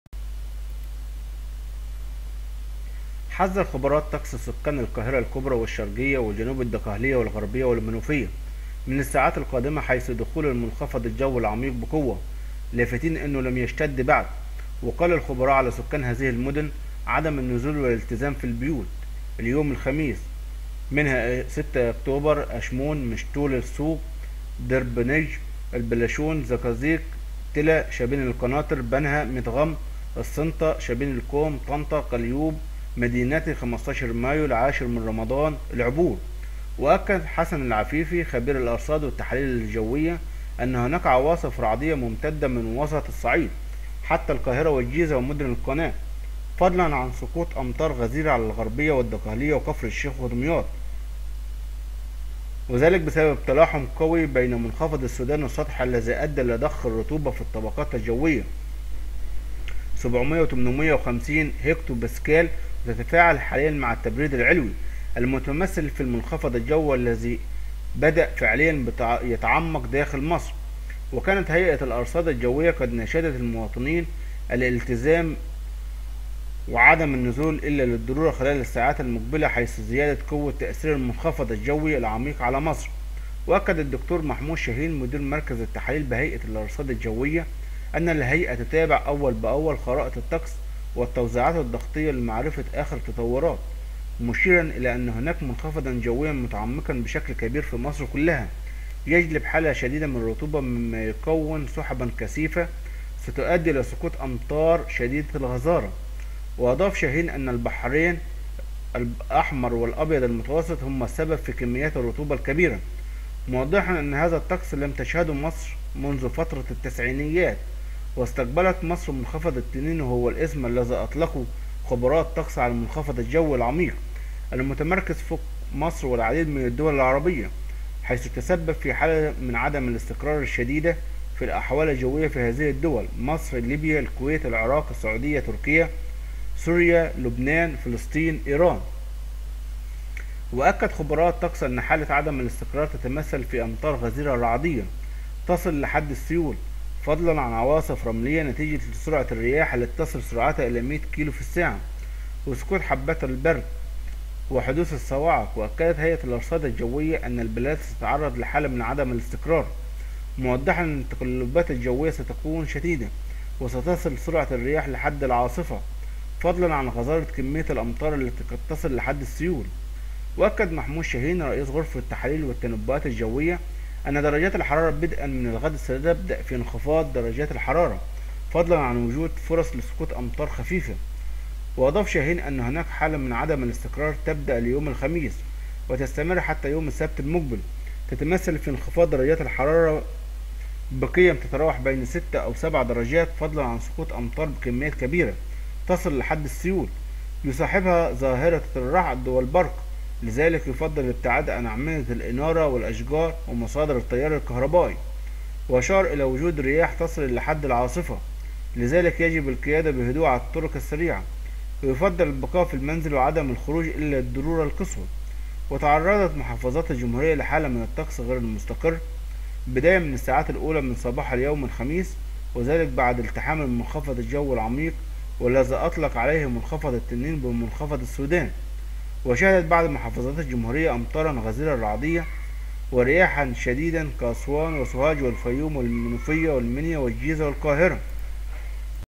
تحذير جديد لسكان هذه المدن بسبب حالة الطقس اليوم
حذر خبراء الطقس سكان القاهرة الكبرى والشرقية وجنوب الدقهلية والغربية والمنوفية، من الساعات القادمة، حيث دخول المنخفض الجوى العميق بقوة، لافتين أنه لم يشتد بعد.
وأكد حسن العفيفى ، خبير الأرصاد والتحاليل الجوية ، أن هناك عواصف رعدية ممتدة من وسط الصعيد حتى القاهرة والجيزة ومدن القناة، فضلا عن سقوط أمطار غزيرة على الغربية والدقهلية وكفر الشيخ ودمياط ، وذلك بسبب تلاحم قوى بين منخفض السودان السطحي الذى أدى إلى ضخ الرطوبة في الطبقات الجوية 700 و850 هيكتوباسكال وتتفاعل حالياً مع التبريد العلوي المتمثل في المنخفض الجوي اللي بدأ فعلياً يتعمق داخل مصر .
وكانت هيئة الأرصاد الجوية قد ناشدت المواطنين، التزام المنازل وعدم النزول إلا للضرورة خلال الساعات المقبلة حيث زيادة قوة تأثير المنخفض الجوى العميق على مصر.
وأكد الدكتور محمود شاهين مدير مركز التحاليل بهيئة الأرصاد الجوية، أن الهيئة تتابع أولا بأول خرائط الطقس، والتوزيعات الضغطية، لمعرفة آخر التطورات، مشيرا إلى أن هناك منخفضا جويا متعمقا بشكل كبير في مصر كلها، يجلب حالة شديدة من الرطوبة مما يكون سحبا كثيفة ستؤدي إلى سقوط أمطار شديدة الغزارة.
وأضاف شاهين أن البحرين الأحمر والأبيض المتوسط، هما السبب فى كميات الرطوبة الكبيرة موضحا أن هذا الطقس لم تشهده مصر منذ فترة التسعينيات.
واستقبلت مصر "منخفض التنين" وهو الاسم الذى أطلقه خبراء الطقس على المنخفض الجوى العميق المتمركز فوق مصر والعديد من الدول العربية، حيث يتسبب فى حالة من عدم الاستقرار الشديدة فى الأحوال الجوية فى هذه الدول "مصر، ليبيا، الكويت، العراق، السعودية، تركيا، سوريا، لبنان، فلسطين، إيران".